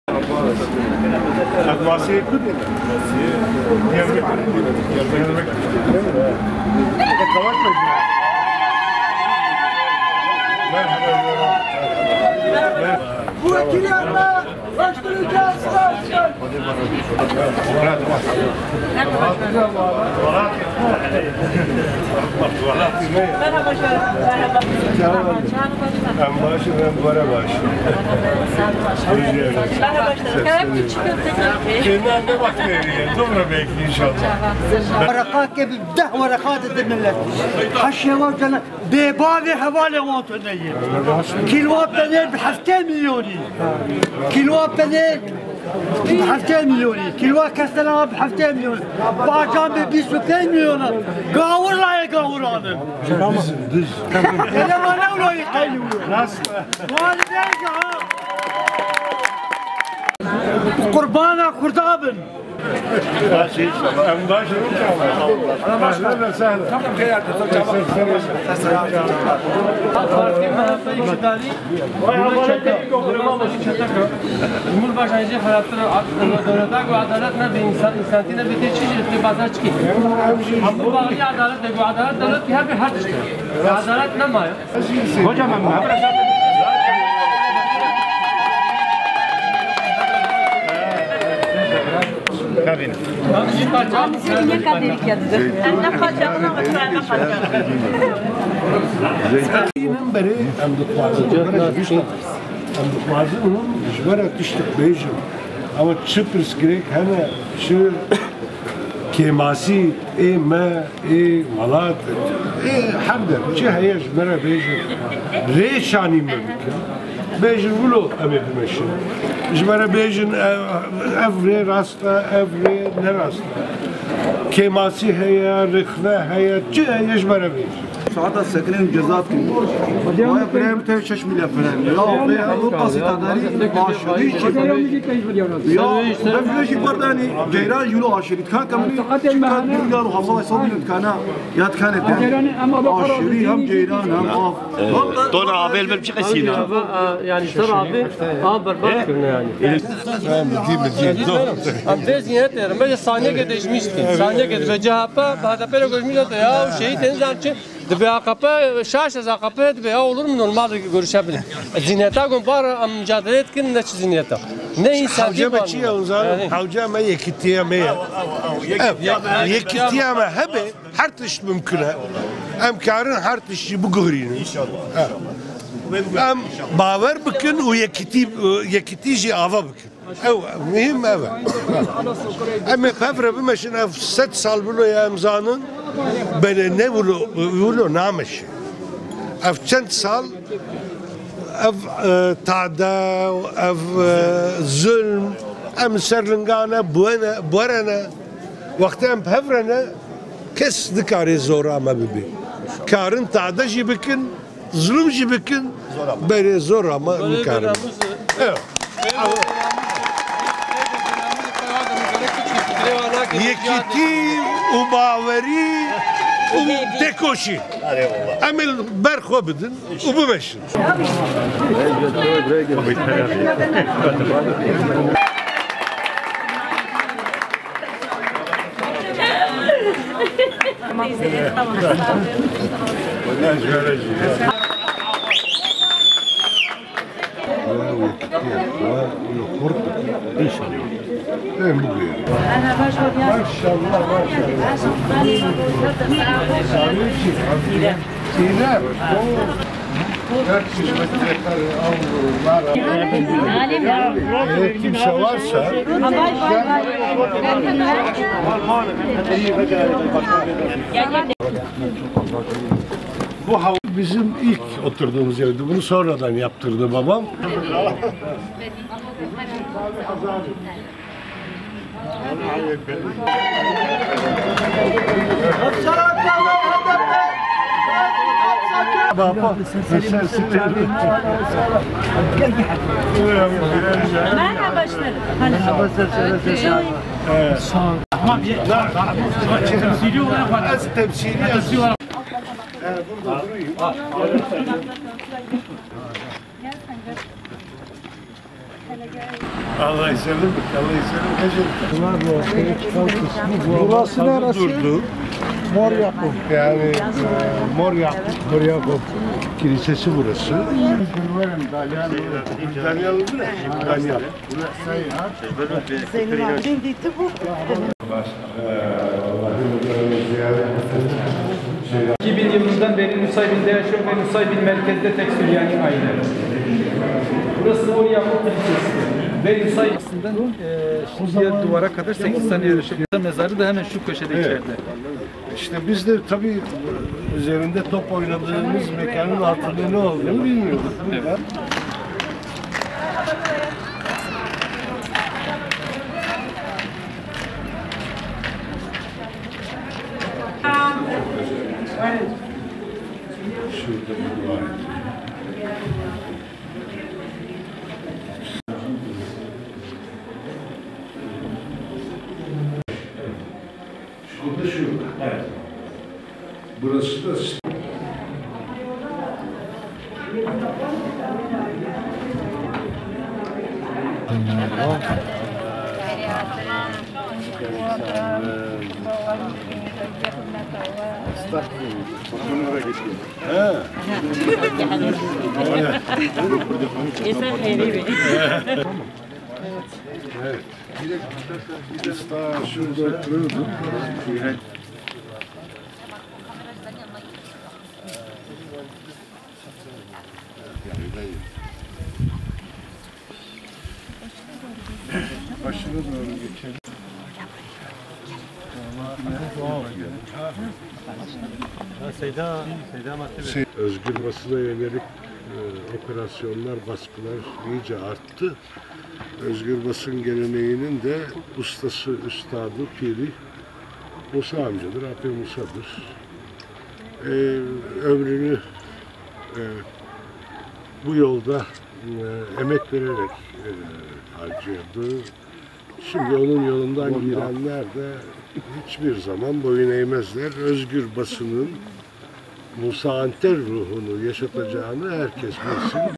SANCAR NİSÊBÎN'DE COŞKUYLA KARŞILANDI
HDP Eş Genel Başkanı Mithat Sancar, Nisêbîn’de esnaf ziyareti yaparak, sorunlarını dinledi. Ardından Apê Musa’nın anıt mezarını ziyaret eden Sancar, “Şimdi O’nun yolundan gidenler de hiçbir zaman boyun eğmezler. Özgür Basın’ın, Musa Anter ruhunu yaşatacağını herkes bilsin” dedi.

Halkların Demokratik Partisi (HDP) Eş Genel Başkanı Mithat Sancar, Mêrdîn'deki programının ikinci gününde Nisêbîn (Nusaybin) ilçesine geçti. Sabah saatlerinden itibaren başlayan program kapsamında Sancar’a HDP Parti Sözcüsü Ebru Günay, HDP Halklar ve İnançlar Komisyonundan Sorumlu Eş Genel Başkan Yardımcısı Tülay Hatimoğulları, HDP Milletvekilleri Pero Dündar ile İmam Taşçıer de eşlik etti. Sancar ve beraberindeki heyet ilk olarak Nisêbîn esnafını ziyaret etti. Zılgıtlar ve alkışlar eşliğinde karşılanan Sancar, esnaf ile sohbet edip, ikram edilen çay ve kahveyi tattı. Ardından Kaçakçılar Çarşısında esnasında sorunlarını dinleyen Sancar ile esnaf arasında ekonomi ve Kürtlerin geleceğine dönük diyaloglar yaşandı.

Bir yurttaş artan gıda fiyatları nedeniyle tepkisini dile getirirken "Peynirin kilosu 70 lira. Domates 25 lira" siteminde bulundu. Sancar ise, sorunları birlikte aşacaklarını belirterek, "Birlikte değiştireceğiz " cevabını verdi. Bir başka esnaf da, Türkiye’de büyük bir adalet sorunu olduğunu dile getirerek, "Adaletin olmadığı yerde hiçbir şey düzelmez. Çözüm için adalet gerekiyor" diyerek tepkisini dile getirdi. Sancar ise, “Bizim mücadelemiz de bunun içindir. Biz istiyoruz ki, bize her sorunu anlatın. Bize yol gösterin. Böyle yürüyeceğiz. Bize doğru yolu da yanlış yolu da söyleyin. Gösterin. Eksikliklerimiz, eleştirileriniz ne varsa söyleyin” dedi.

ESNAFI DİNLEDİ, ÇÖZÜMÜ SÖYLEDİ: BİRLİĞİMİZİ KURALIM

Bir başka esnaf da Nisêbîn Qamislo Sınır Kapısının kapalı olmasından dert yanarken, bir başka esnaf ise yazılan cezalarla, faturalarla baş edemediklerini ifade etti. Esnaflar iktidar ve muhalefet partilerinin Kürt siyasetine yaklaşımına dair de değerlendirmelerde bulunurken, Adalet Bakanlığı heyetinin HDP’ye yaptığı ziyaret üzerinde yapılan tartışmalara tepki göstererek, “Bizim mücadele ettiğimiz zihniyet zihniyet değil” tepkisinde bulundu.

Bunun üzerine Sancar, “Birliğimizi oluşturmamız lazım. Biz birlik olursak her şey mümkün. Her şeyi de değiştirebiliriz. İnanalım ve birliğimizi kuralım. Birlik, inanç ve mücadele. Direneceğiz, devam edeceğiz” cevabı verdi.

TARİHİ MEKANLARI GEZDİ

Ziyaretine devam eden Sancar, doğduğu evi ve “Kürtçeyi burada öğrendim” dediği sokakları gezerek devam ederken ardından kapatılan Tarihi Nusaybin Tren Garına geçti. Burada incelemelerde bulunan Sancar, ardından ise Tarihi Mor Yakup Kilisesine geçerek, burada kazı çalışmalarıyla ilgili bilgi aldı. Sancar’ın sonraki durağı Zeynelabidin Türbesi olurken, burada dua okuyan Sancar, ardından ise programının son durağı olan Sitîlîlê (Akarsu) kırsal mahallesine geçti.

‘ÖZGÜR BASIN BOYUN EĞMEZ’

İlk olarak Kürt bilgesi Apê Musa’nın (Musa Anter) anıt mezarını ziyaret eden Sancar, burada kısa bir konuşma yaptı. Özgür Basına yönelik operasyonlar ve baskıların iyice arttığını kaydeden Sancar, “Özgür Basın geleneğinin de ustası, piri, Apê Musa’dır. Ömrünü bu yolda emek vererek, harcadı. Şimdi O’nun yolundan gidenler de hiçbir zaman boyun eğmezler. Özgür Basın’ın, Musa Anter ruhunu yaşatacağını herkes bilsin. Bu vesileyle de bunu tekrar hatırlatalım. Hapishanede tutulan Özgür Basın geleneğinden gelen herkese de buradan Musa Anter’in mezarının başından , Apê Musa’nın mekanından selam gönderiyoruz” dedi.

Ardından köyde esnaf ve yurttaşlarla bir araya gelen Sancar ve beraberindeki heyet, programını sonlandırdı.